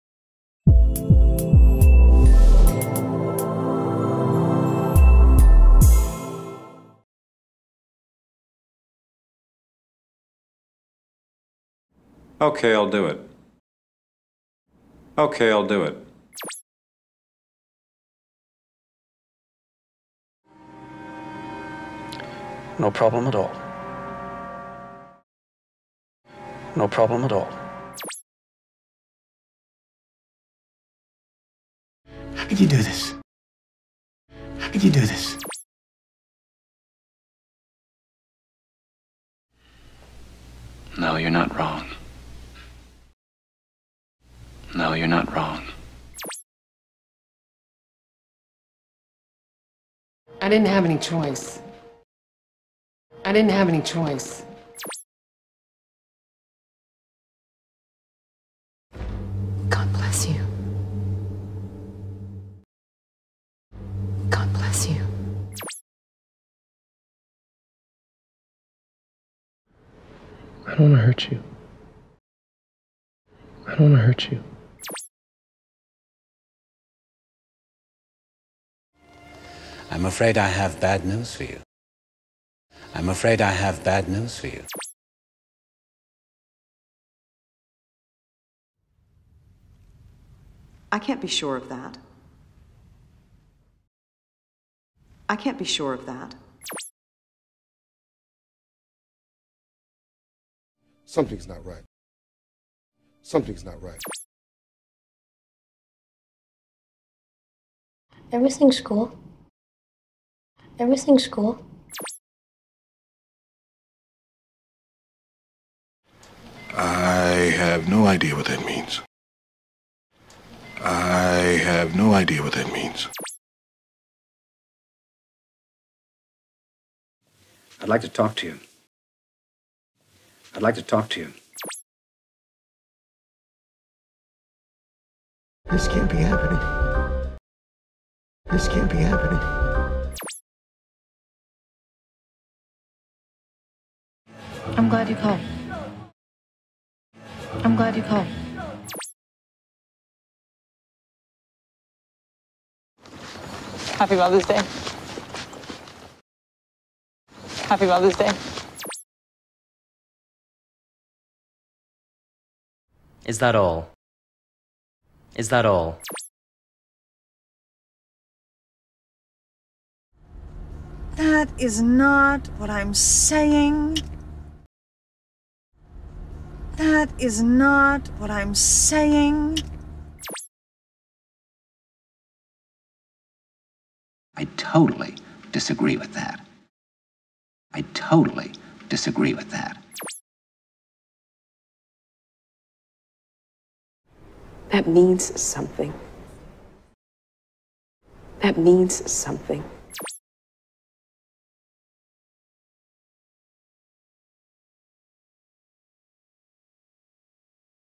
English in 3 minutes (Pre-Intermediate / A2/B1) - Speaking: Useful everyday phrases 29
Here comes another video for pre-intermediate students including various film excerpts to illustrate the use of everyday phrases. :)